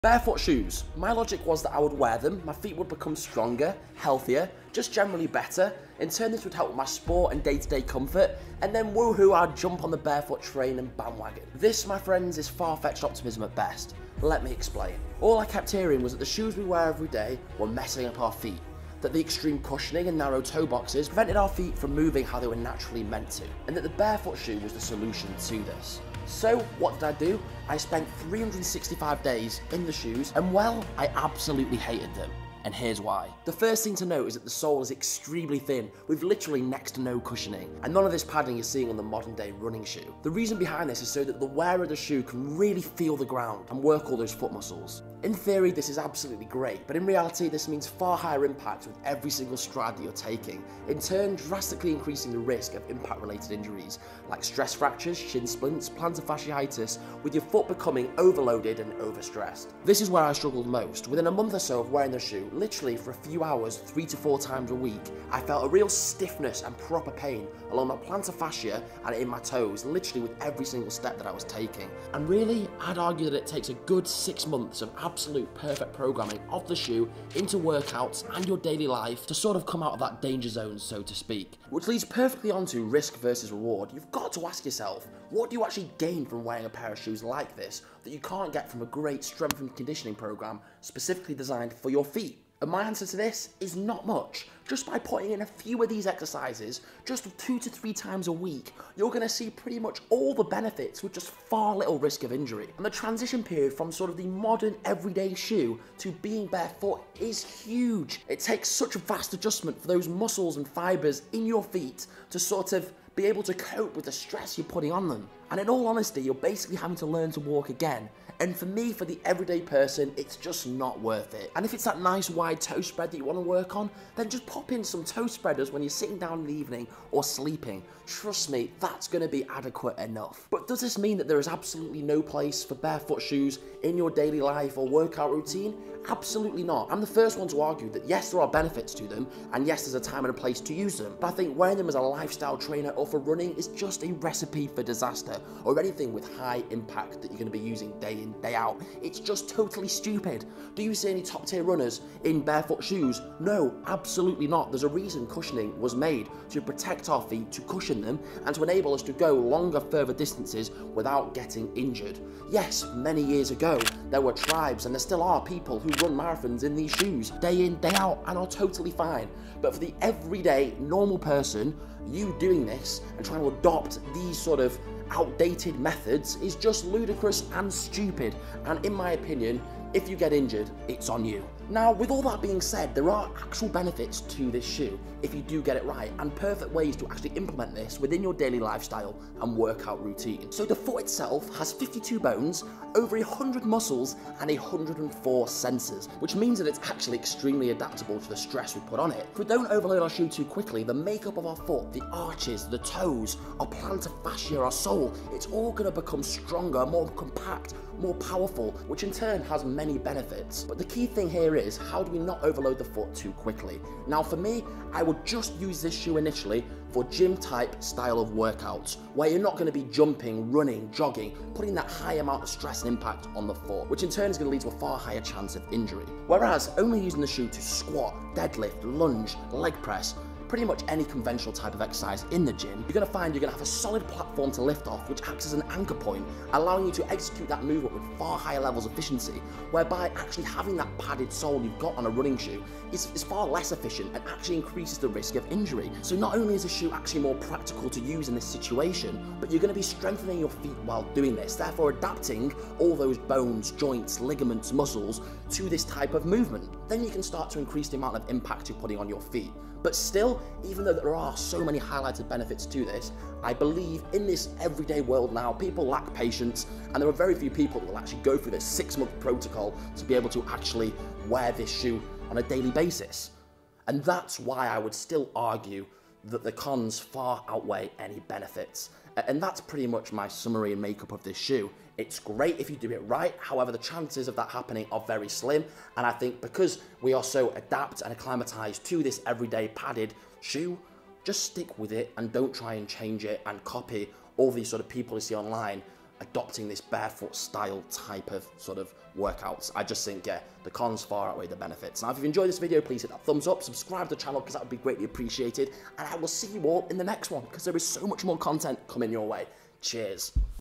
365 DAYS in Barefoot Shoes.... My Feet Are Broken, The TRUTH Why I Hate Them !
The shoes that we wear everyday are apparently causing a ton of pain throughout our body (feet, back, ankle, and neck pain), because they don't allow us to walk how we're designed to. In this video we explore the history of shoes and how barefoot shoes in my opinion actually have not solved the problem, allowing us to walk naturally, having stronger more pain free bodies, but instead for the everyday common person increase the risk of injury and actually possess far more downside to any benefit. a response to @heymikehanna who put up a really great argument for the benefits of this shoe. Basically this is my response to this !

FOR Strengthening your feet without actually having to risk wearing barefoot shoes !